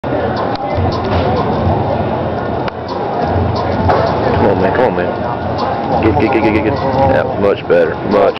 Trenton Calvert 5-10-5 Drill #1
Trenton Calvert of Spencer County Middle School in Taylorsville, KY - first attempt at 5-10-5 drill at the Junior Rank Combine in Chicago - 7th grade division.